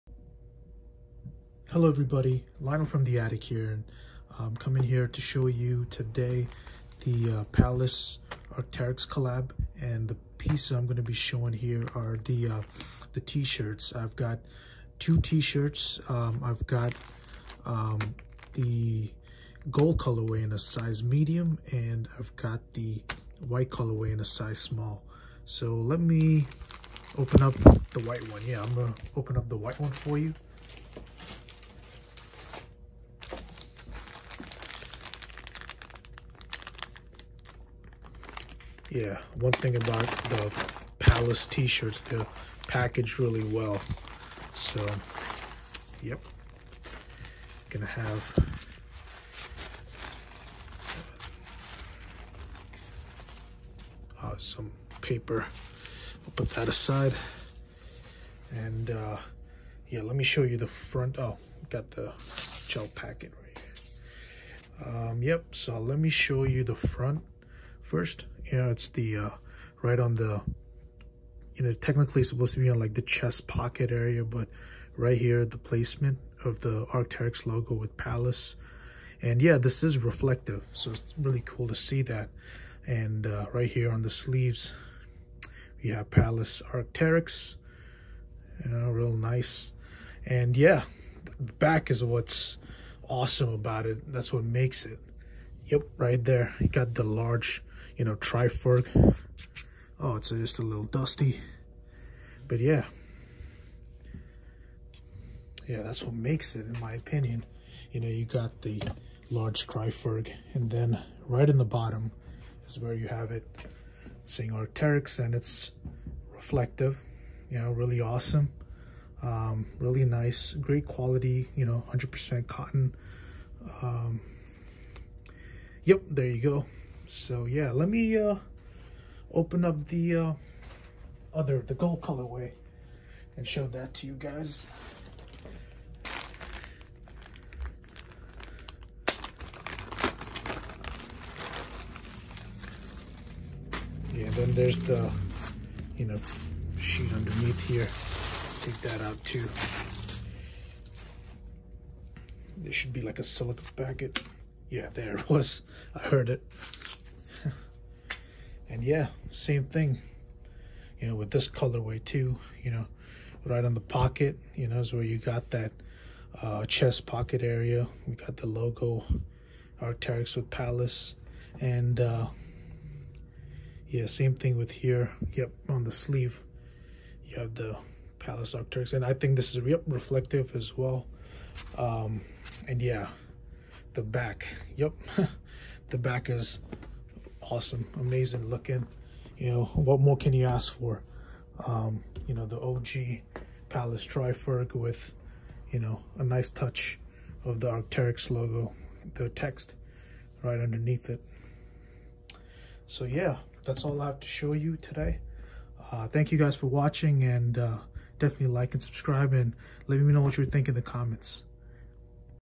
Palace Arc’Teryx T-Shirt
In this video we check out two colorways of the Palace Arc'Teryx T-Shirt. The colorways we check out in this video are white and gold.